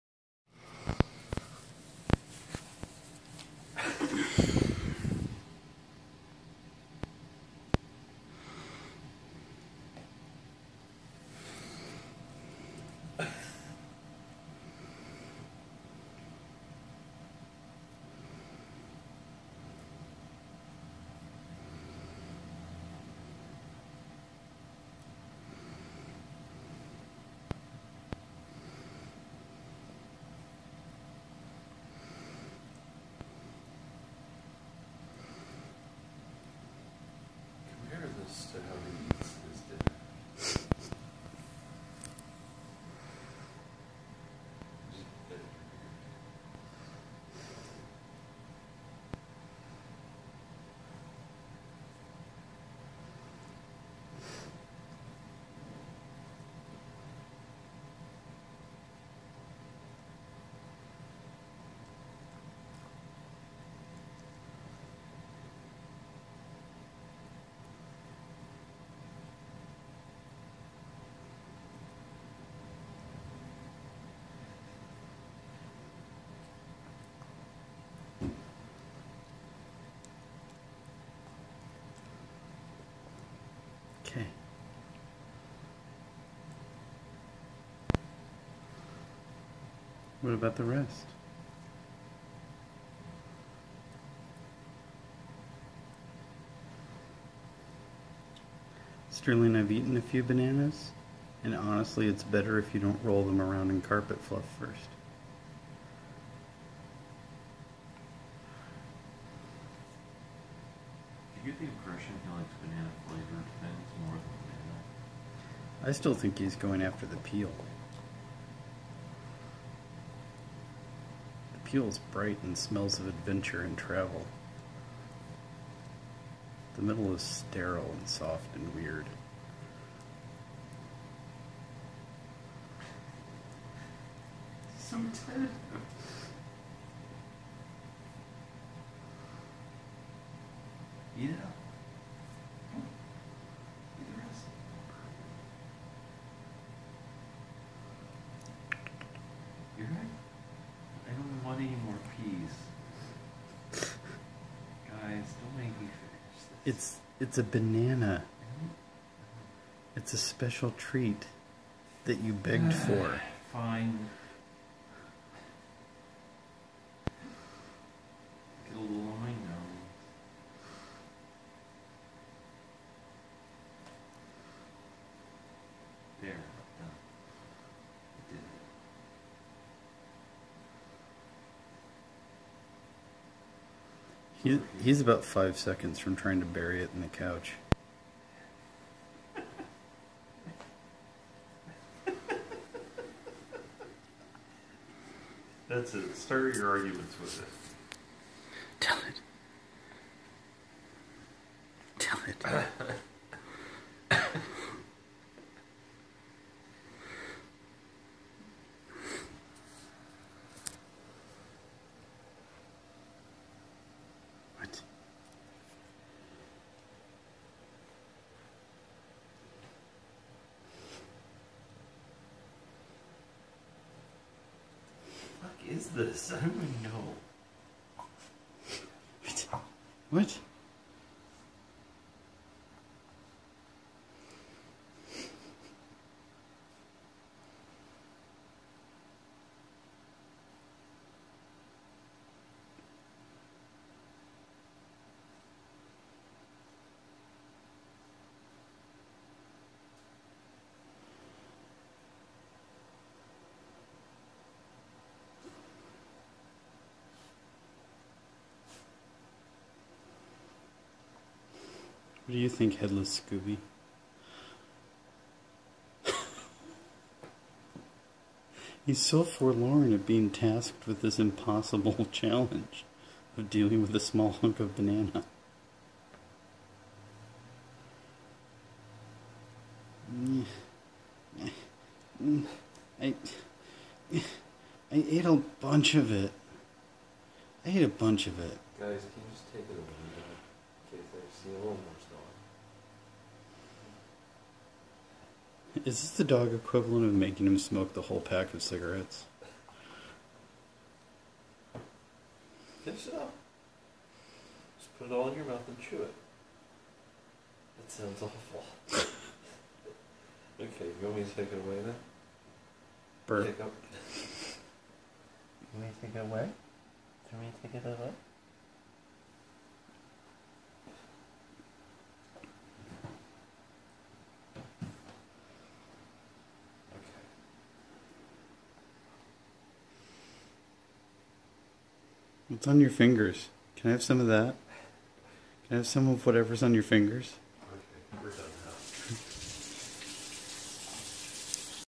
Sterling vs Banana
After days of begging, and confirmation from multiple sources that bananas are not toxic to puppies, we finally offered Sterling the prize he'd been yearning for.

FYI our carpet is not festooned with random filth, that's just detritus from recently eviscerated soft toys.